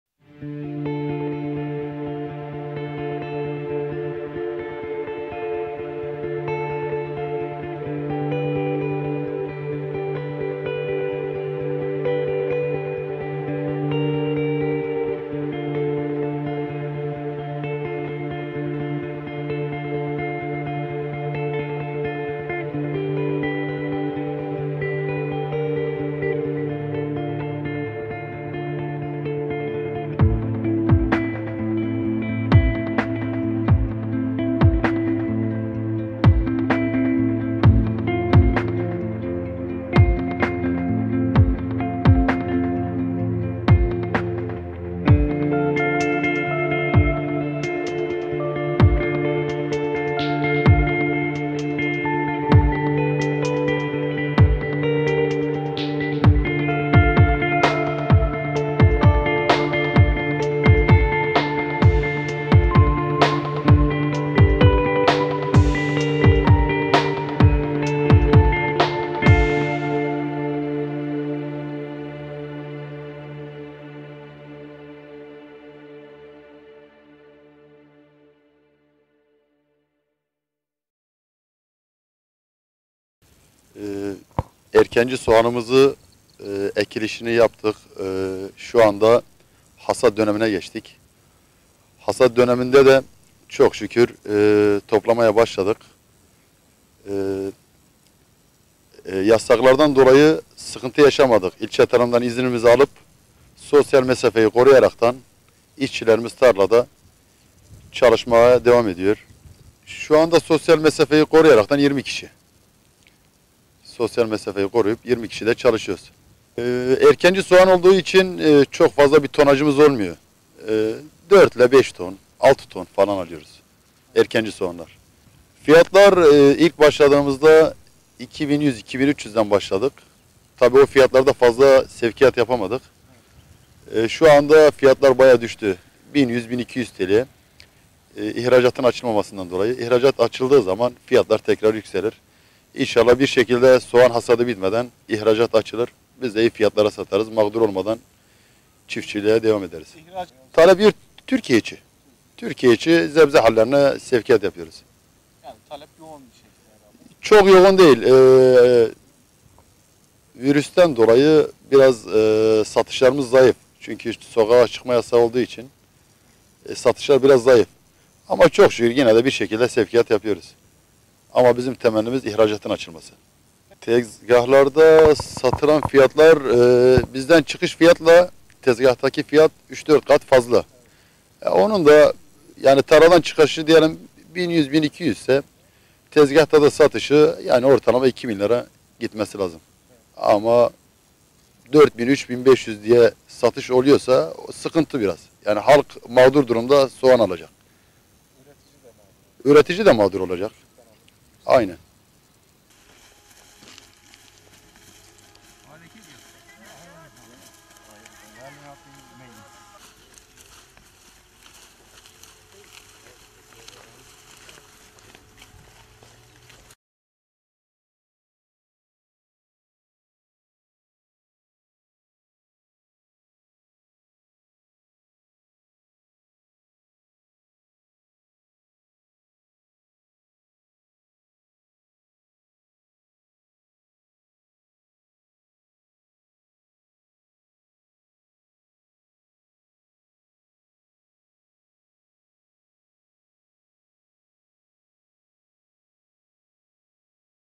Hatay’da Turfanda Soğan Hasadı Başladı
Hatay’ın en verimli topraklarının bulunduğu Amik Ovası'nda 20 bin dekar alanda üretimi yapılan turfanda soğan hasadı başladı. Hasat mayıs ayının sonuna kadar devam edecek.

Hatay’da Amik Ovası’nda gerçekleştirilen turfanda soğan hasadı başlarken, mayıs ayının sonuna kadar devam edecek. Hatay genelinde 20 bin dekar araziye ekilen soğanlardan dekar başına 3 ile 5 ton arasında ürün elde ediliyor. Ülkenin farklı illerinden gelen tarım işçileri sökümünü yaptıkları soğanları kestikten sonra çuvallara doldurarak sevkiyata hazır hale getiriyor. Daha sonra kamyonlara yüklenen soğanlar Türkiye'nin farklı bölgelerine gönderiliyor.
Amik ovasındaki üreticilerden Yıldırım Doğu, soğanın ortalama 1 lira 10 kuruştan tarladan satıldığını belirterek “Erkenci soğanın ekimini yaptık ve şimdiler de hasadını yapıyoruz. Hasat dönemi de iyi gidiyor, toplamaya başladık. İşçi konusunda İlçe Tarım Müdürlüğü’nden izin aldığımız için kısıtlamadan dolayı bir sıkıntı yaşamadık. Sosyal mesafeyi koruyarak işçilerimiz tarlada çalışmaya devam ediyor. İzin verilen sayı 20 işçi. Erkenci soğan olduğu için çok faza tonajımız olmuyor. Ortalama 4-5 ton ürün alabiliyoruz. Hasadın ilk günlerinde 2 lira 10 kuruş idi ama o fiyatlarda fazla sevkiyat yapamadık. Şu anda ise fiyatlar 1 lira 10 kuruşa satılıyor, ihracatın kapalı olmasından dolayı. İhracat açıldığı zaman fiyatlar yeniden yükselir. Soğan hasadı bitmeden ihracat inşallah açılır bizde mağdur olmadan iyi fiyata satarız” dedi.
Genel olarak Türkiye içi sebze hallerine gönderdiklerini belirten Doğu tarladan çıkış fiyatı ile tezgahta ki fiyatların çok farklı olduğunu belirterek “virüsten dolayı satışlarımız biraz zayıf. Bizim temennimiz ihracatın açılması. Bizim sattığımız fiyat ile tezgahta ki fiyat 3-4 kat fazla. Tarladan çıkışı 1 lira 10 kuruş olan soğanın tezgahta ortalama 2 liradan satılması lazım. Ama 4 lira gibi rakamlara satılırsa, halkımız da üretici de mağdur olur” ifadelerine yer verdi.